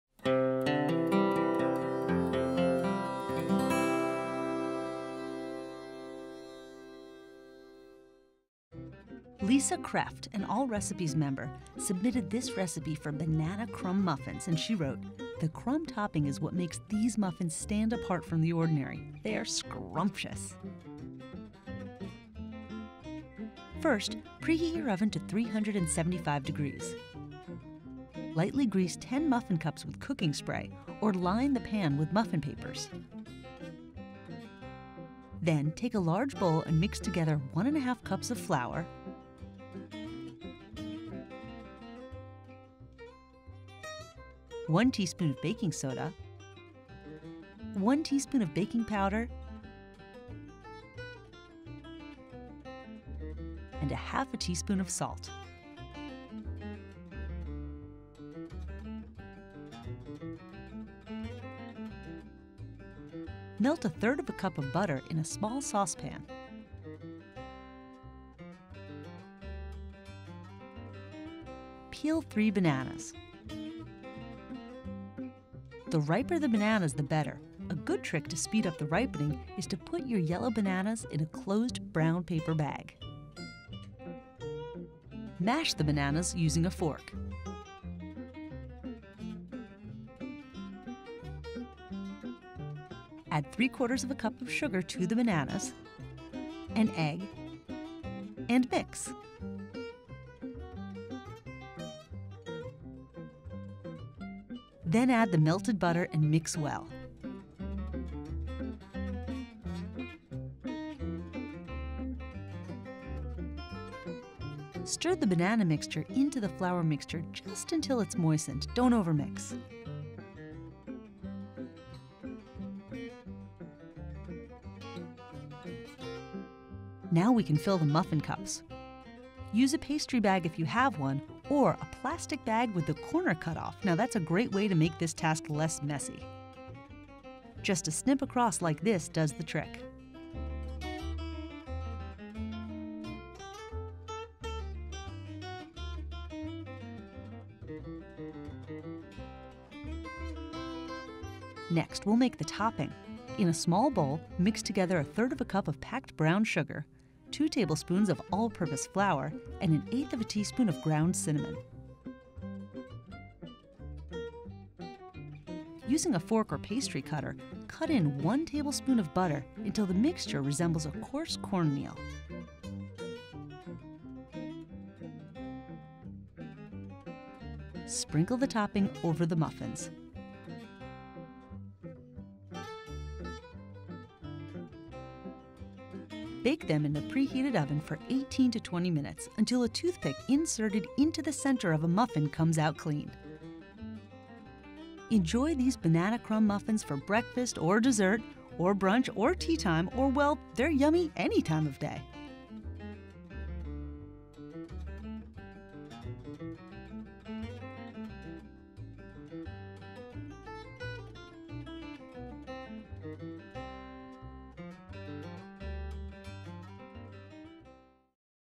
How to Make Banana Crumb Muffins | Allrecipes.com
Watch how to make top-rated banana muffins, made special with a crunchy topping. Positively addicting!